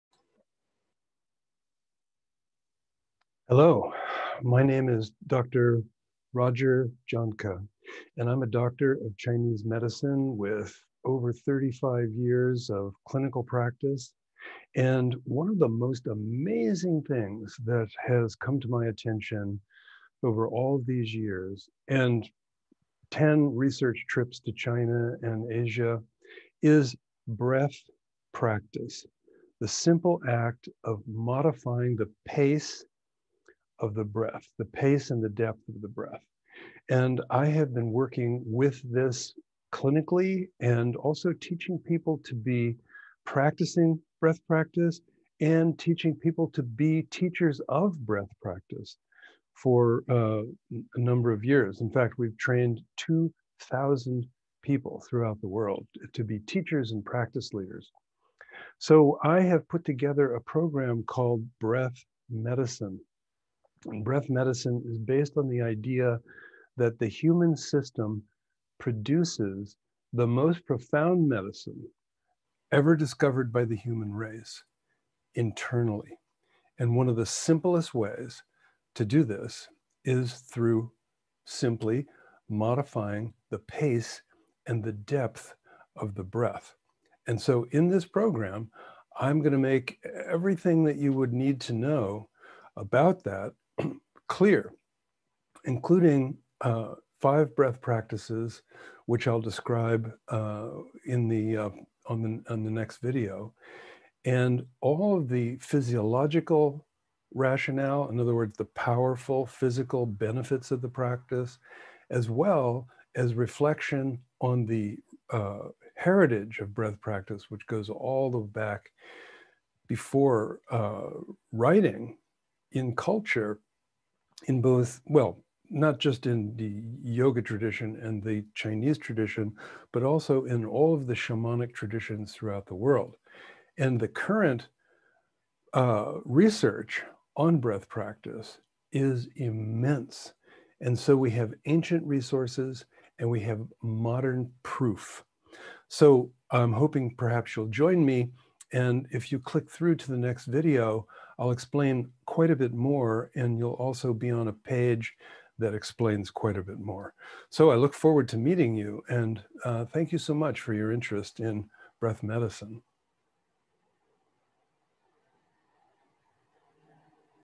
Breath Medicine Introduction 1 by Dr. Roger Jahnke, OMD
Breath Medicine - Activate the Most Profound Medicine
by Dr. Roger Jahnke Author of The Healer Within and The Healing Promise of Qi

Inhale, Exhale, There's Where the Power Is!
"The breath is the most powerful tool for gathering, circulating, purifying, and directing inner functional resources the natural homeostatic capacity according to biological science."